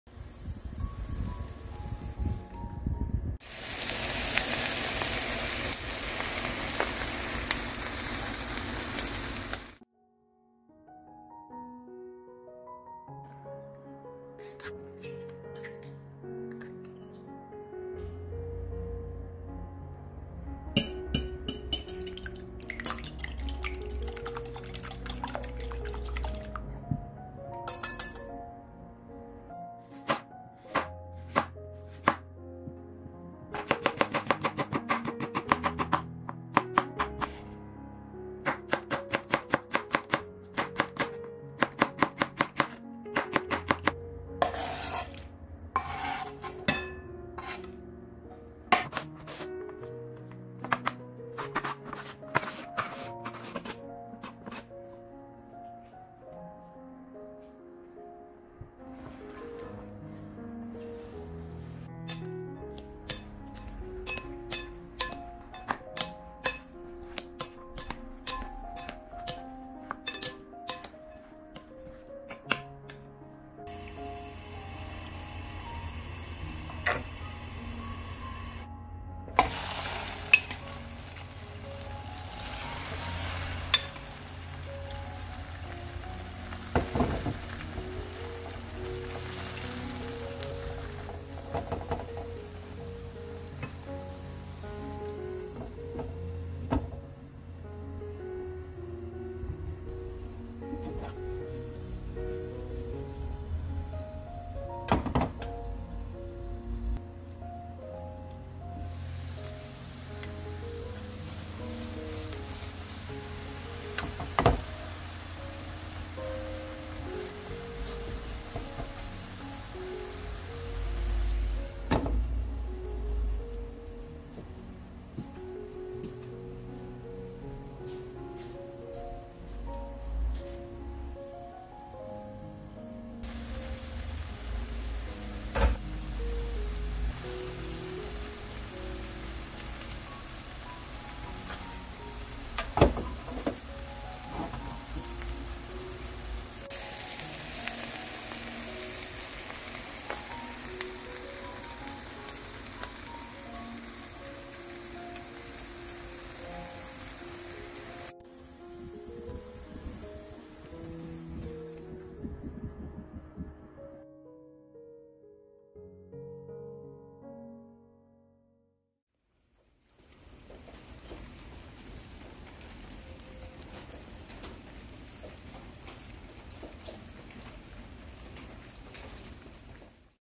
호박전만드는법 밀가루 없이 호박전 맛있게 부치는 방법
호박전만드는법
밀가루 없이 호박전 맛있게 부치는 방법

재료
둥근호박 1개
계란 3개
당근 4조각
청양고추 2개
소금 1찻술
식용유

호박은 씨앗부분은 사용하지 않음
(버리지 말고 된장 찌개 넣어 드세요)